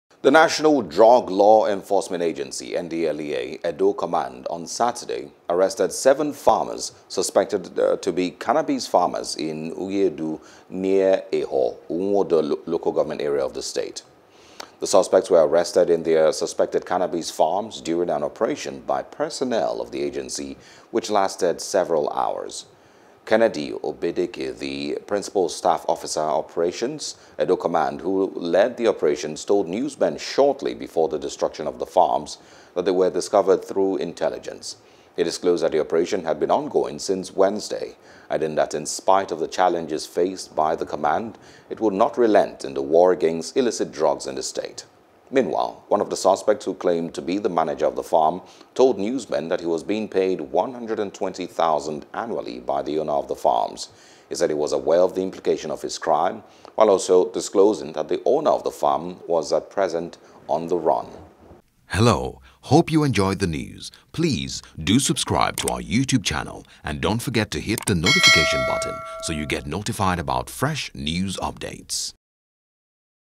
NDLEA Destroys 22 Hectares Of Cannabis Farms | NEWS
The National Drug Law Enforcement Agency (NDLEA)  Edo Command on Saturday arrested seven  farmers suspected to be cannabis farmers in Ughieudu, near Ehor, Uhumwonde Local Government Area of the state.